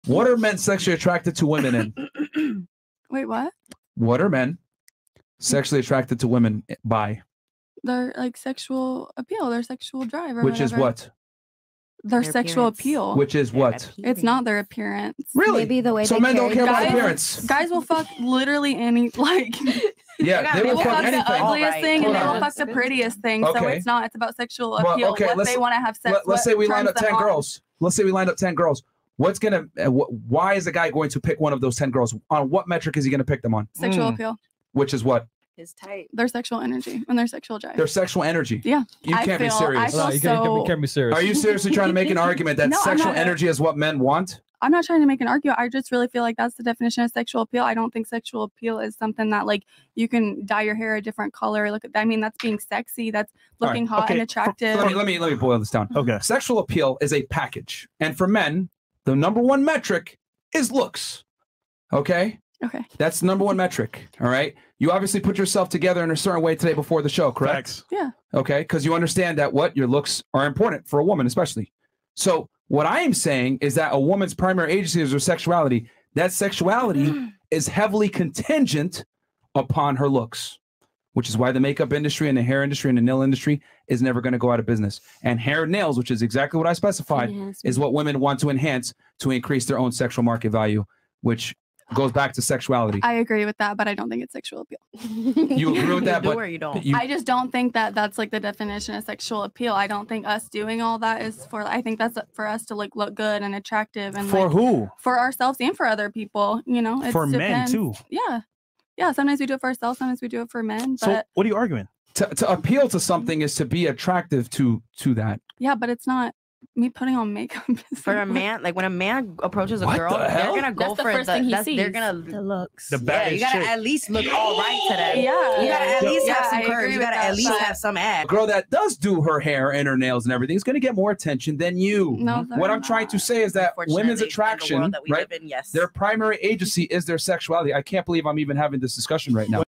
She says HAIR, NAILS AND MAKE UP don't ATTRACT MEN
Clips Channel
➜    / freshandfitclips